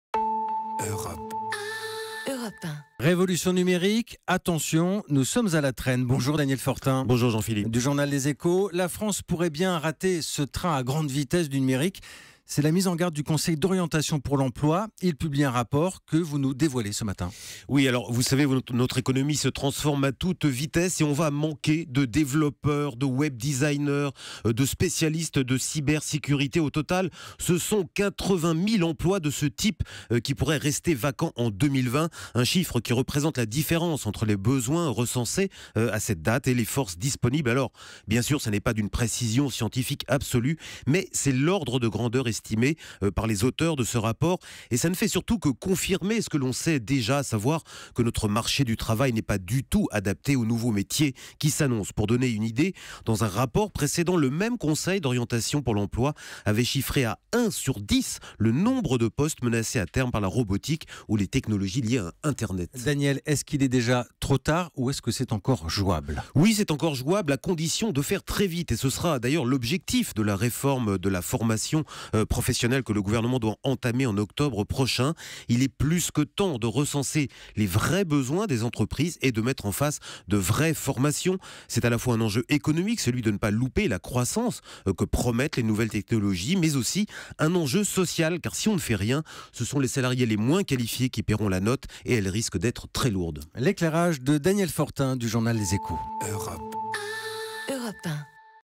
La France pourrait bien rater son virage numérique
Chaque jour, dans son édito, Daniel Fortin du journal Les Echos revient sur un fait précis de l'actualité économique.